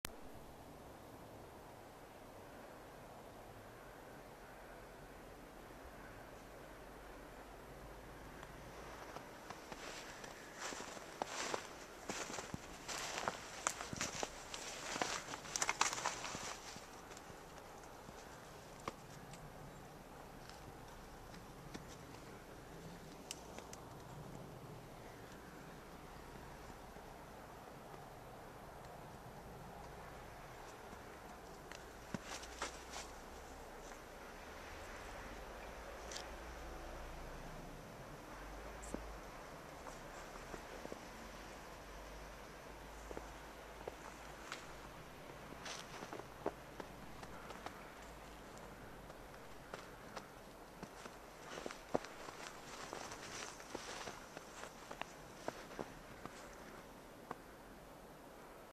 Test Flip UltraHD: Expozice, kresba, fixfokus
Test minikamerky Flip UltraHD (720p). V tomto testu kresba scény, rychlost měření při nejvyšším kontrastu (slunce/stín) a hranice fixfokusu. Je patrné, že rychlý pohyb je největší nepřítel. MP4 soubor měl 70 MB!